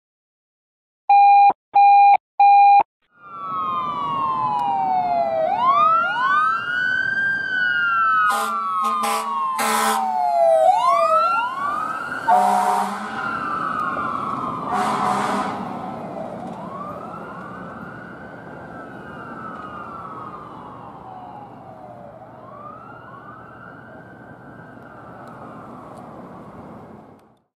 *AIR HORN* Pasadena Fire Dept. RA34 Responding
RA34 is seen here responding to a medical call with Truck 31. Special thanks to the AO for the air horn!

Pasadena, CA - February 2023